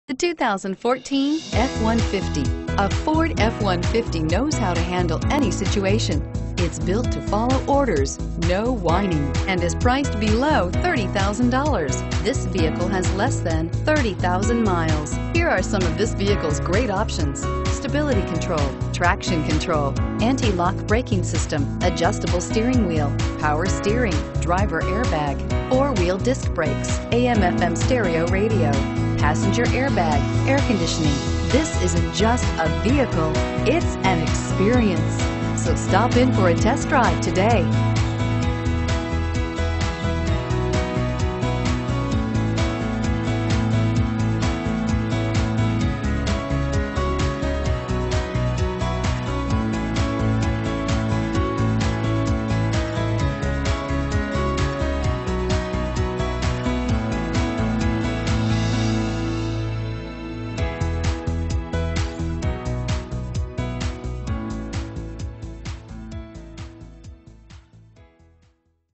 2014 Ford F-150 15T595A
2014 FORD F-150 Crestview, FL
Stock# 15T595A

4060 S. Ferdon

Look at this 2014 Ford F-150 . It has a Automatic transmission and a Regular Unleaded V-6 3.7 L/228 engine. This F-150 has the following options: Variable Intermittent Wipers, Urethane Gear Shift Knob, Transmission: Electronic 6-Speed Automatic -inc: tow/haul mode, Transmission w/Oil Cooler, Towing w/Harness and Trailer Sway Control, Tires: P235/75R17 BSW A/T (5), Tailgate Rear Cargo Access, Steel Spare Wheel, Single Stainless Steel Exhaust, and Side Impact Beams. Stop by and visit us at Hub City Ford, 4060 S. Ferdon, Crestview, FL 32536.